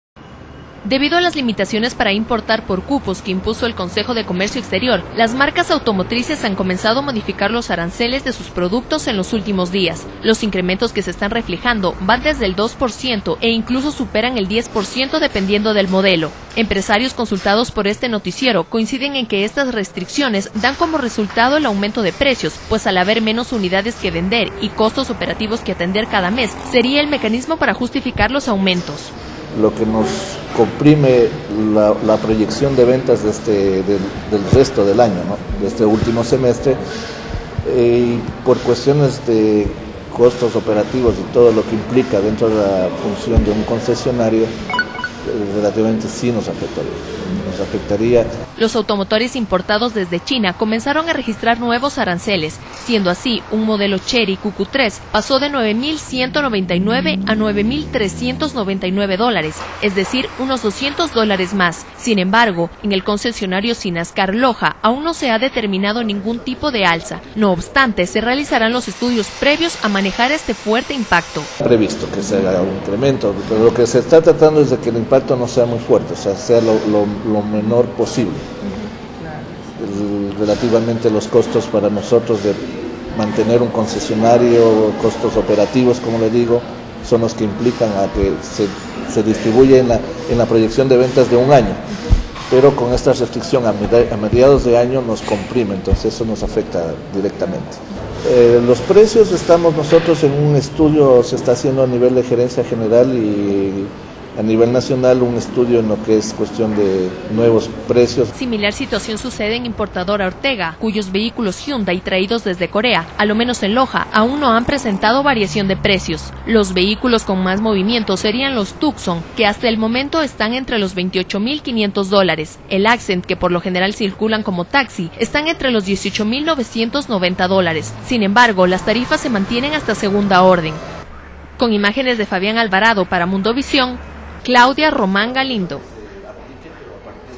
PRECIO CARROS
Con las restricciones por parte del gobierno en cuanto a la importación de vehículos, el precio de estos se alteraría y por ende su comercialización. Muchos concesionarios se ven obligados a alterar el número de vehículos que ingresan al país lo que ocasiona una baja en las ventas. ¿Pero cómo se muestra la situación el Loja? En la siguiente nota se lo mostramos